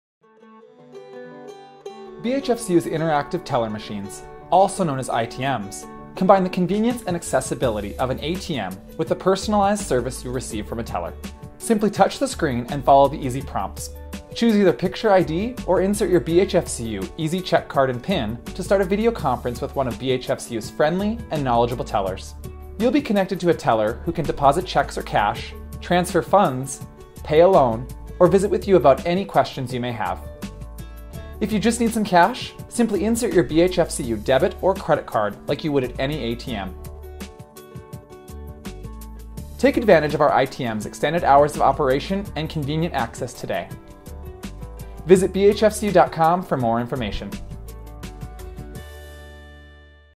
Black Hills FCU | ITM Tutorial
Black Hills Federal Credit Union's new Interactive Teller Machines (ITMs) give you an easy and secure way to handle your finances. Use the ITM's convenient video conferencing option to talk with one of our friendly and knowledgeable tellers about your BHFCU accounts and loans, all from the comfort of your vehicle.